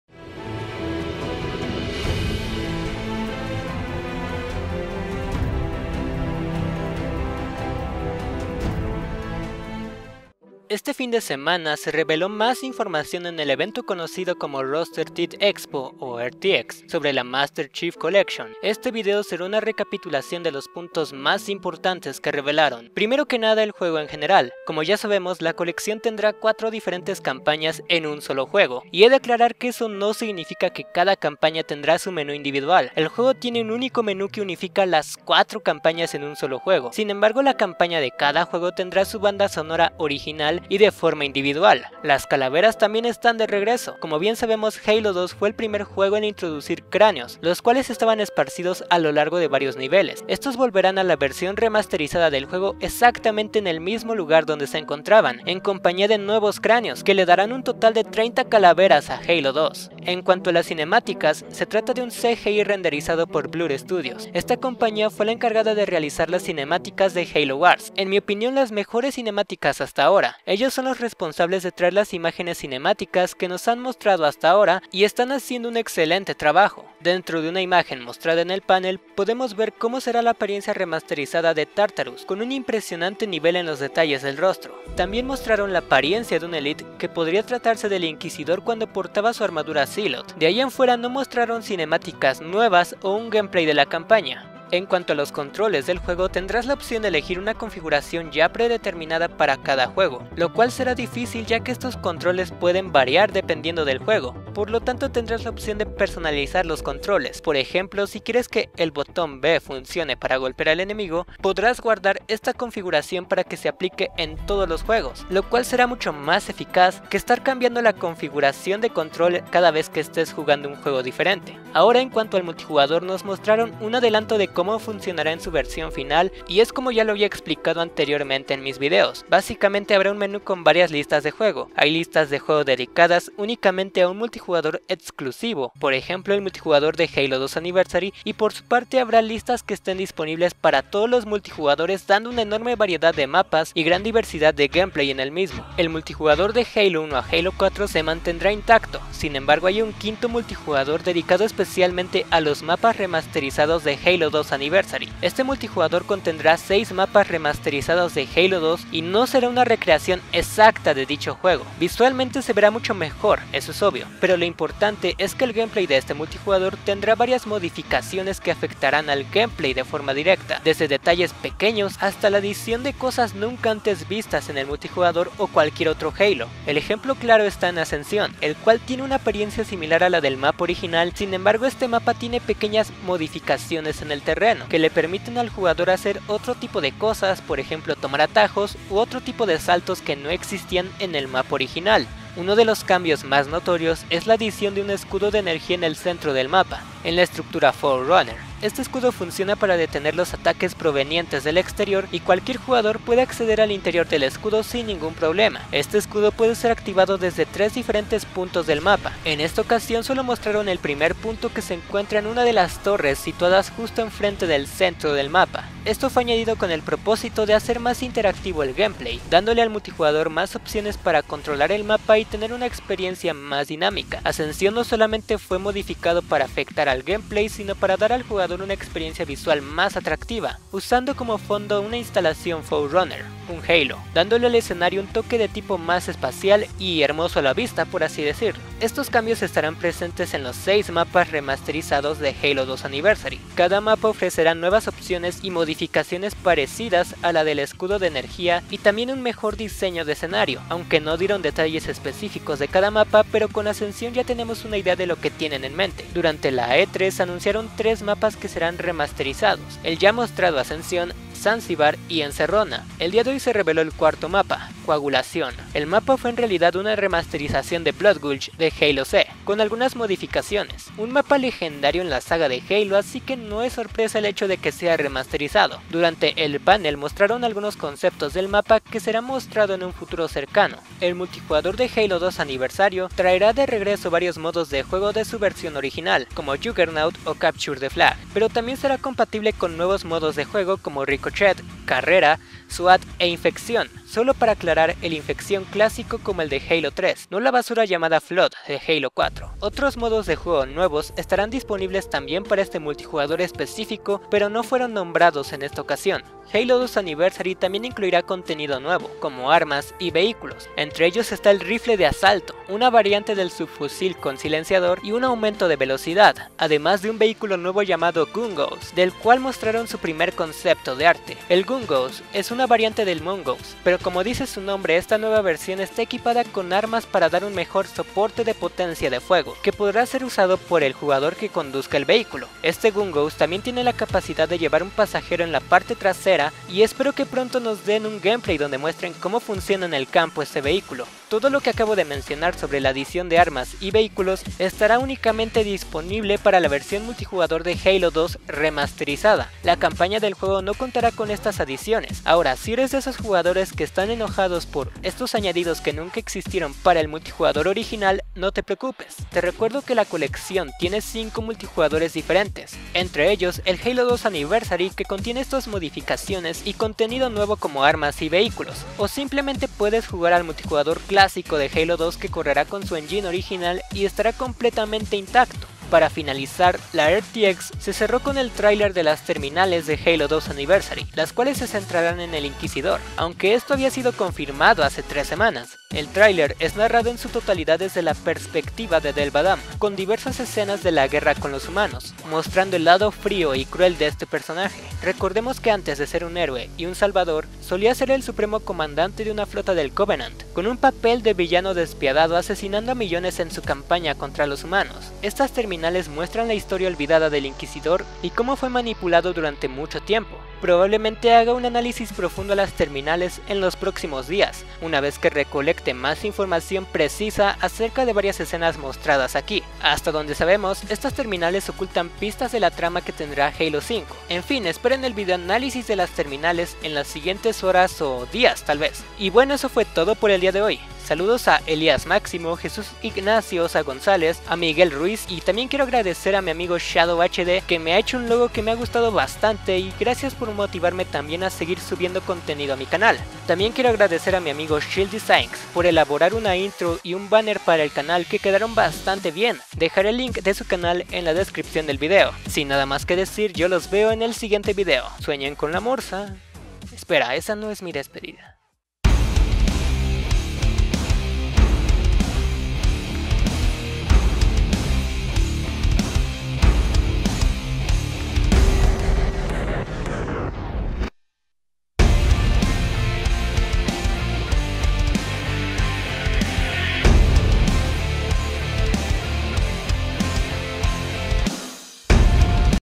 Halo TMCC: Resumen RTX | Gungoose y nuevo contenido para Halo 2A
Recopilación sobre la información de la TMCC (The Master Chief Collection) durante la RTX, incluye vehículos y armas nuevas en Halo 2 Anniversary como la Gungoose, rifle de asalto, subfusil, nuevas modificaciones en los mapas etc.